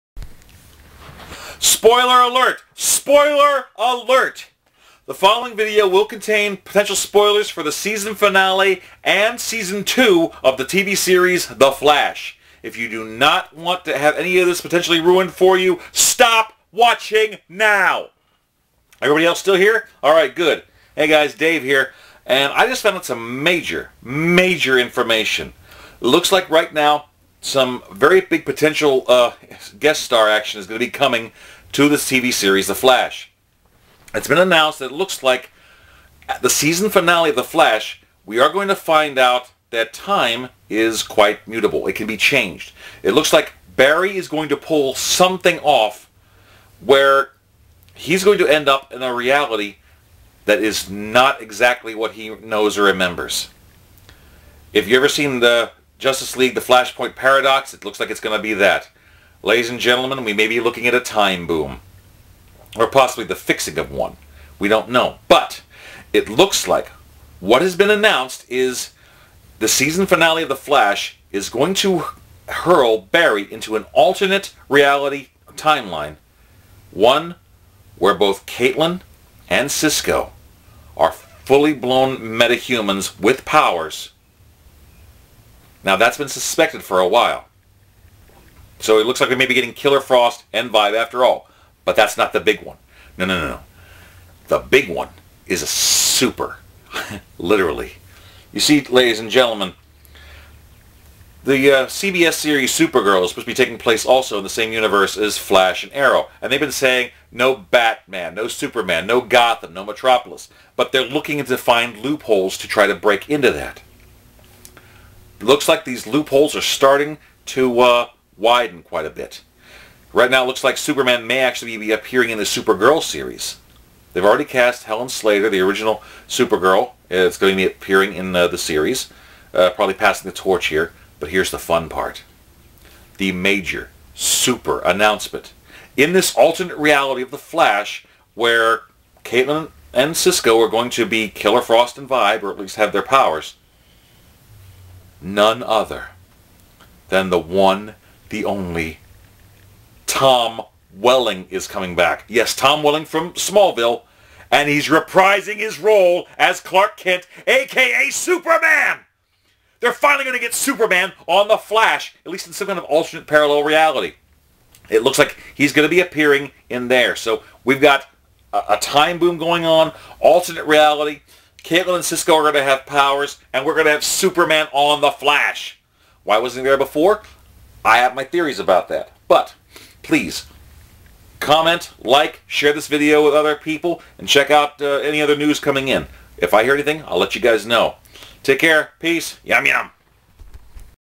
The Flash: Super Season Spoilers!
Some major spoilers have been announced! Stop watching now if you don't want to see!

In The Flash's season finale, Barry will travel to an alternate reality where Caitlin and Cisco will be metahumans. But more than that? The promise that Superman will NOT appear in Arrow of The Flash will be over! Because in that reality, Barry may encounter Superman played by...TOM WELLING! This is slated for season 2. What other news have you heard? Please post here!